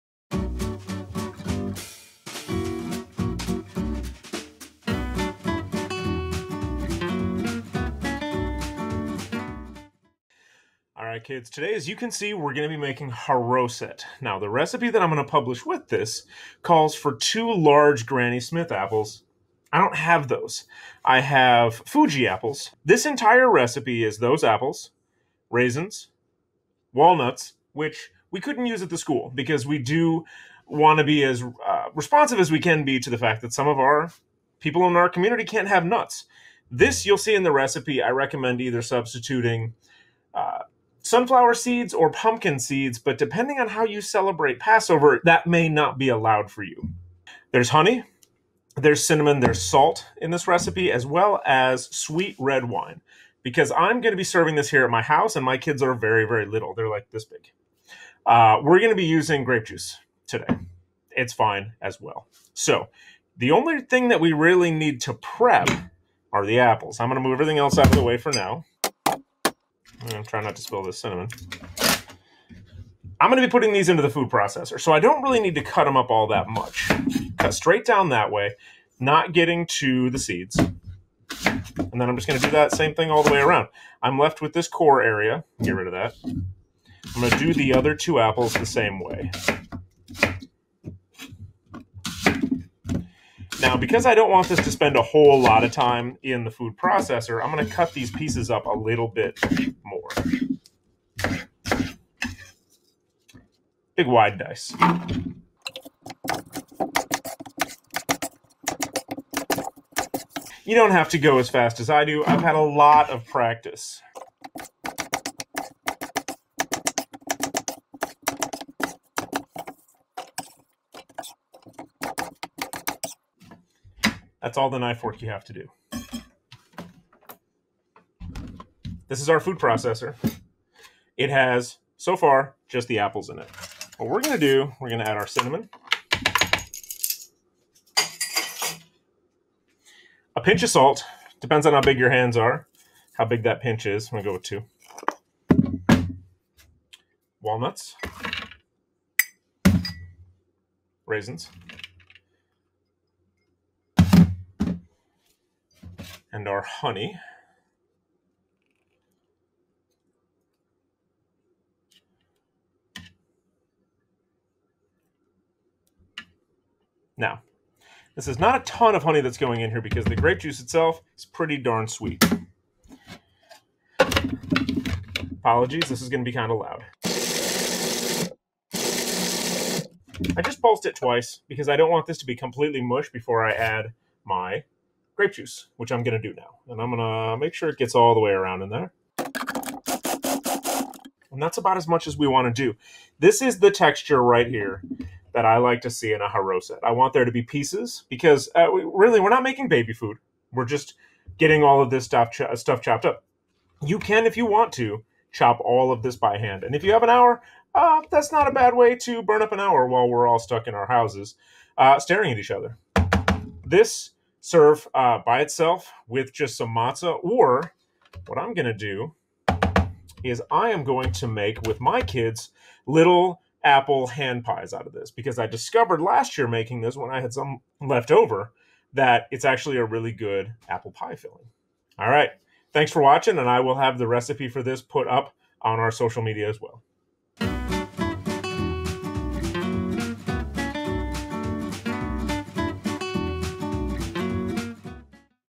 Charoset for Passover
This is a traditional Passover dish you can easily make yourself, and the leftovers make a great filling for apple pie.

WARNING: This recipe contains nuts. If you’re making it for someone with a nut allergy, check the substitution recommendations below.

Ingredients:
2 large Granny Smith apples
1/4 teaspoon ground cinnamon
1/8 teaspoon salt
2 cups toasted walnuts
1/2 cup dark raisins
1 1/2 teaspoons honey or granulated sugar
1/3 cup kosher sweet wine

Preparation:
Step 1: Slice the apples and remove the cores.
Step 2: Place the apples, cinnamon, salt, walnuts, raisins, and honey, into the bowl of a food processor.
Step 3: Pulse once or twice to mix and begin chopping.
Step 4: Add the wine and pulse until the consistency is how you prefer it.
Step 5: Taste. Depending on your wine of choice, this may need a little bit more honey.

Substitutions:
1: To avoid nut allergies, you can use toasted pumpkin seeds or sunflower seeds to achieve the same texture, but during Passover (depending on how you celebrate), seeds like this may be considered kitniyot, and you wouldn’t be able to use them.
2: If you don’t want to use wine, red grape juice is a fine substitution.